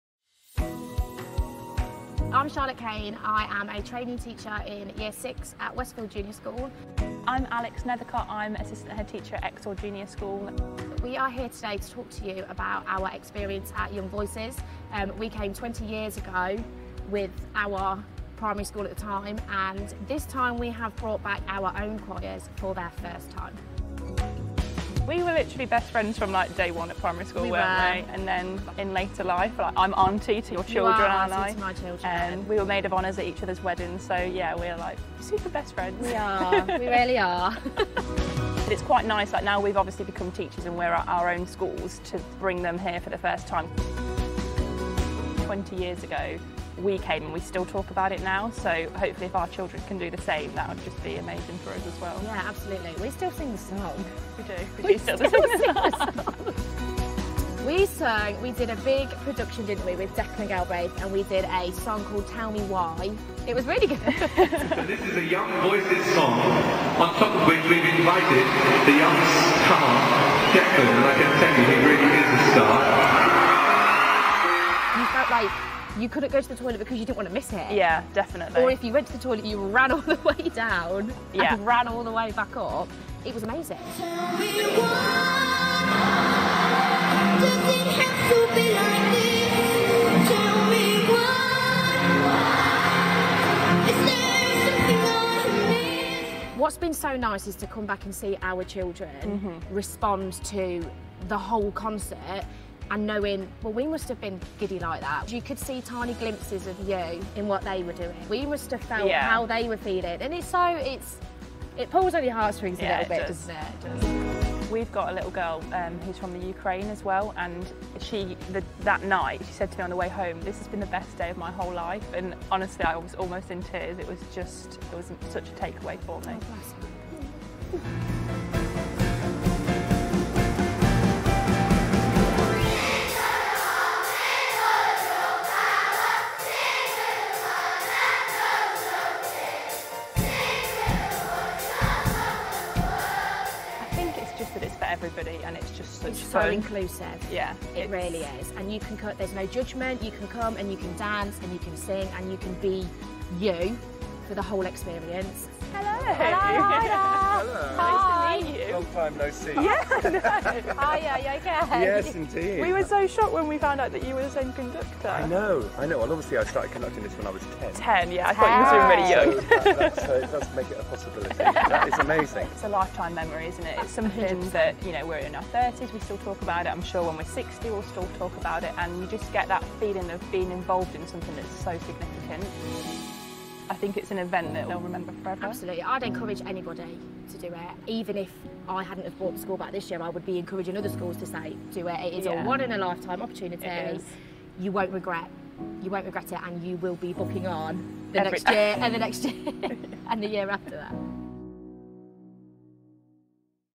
20 Years On - Re-living Childhood YV Memories!
Charlotte Kane from Westfield Junior School and Alex Nethercot from Exhall Junior School tell us about their memories of taking part in Young Voices 20 Years ago and how special it is to come back as teachers bringing their own school choirs.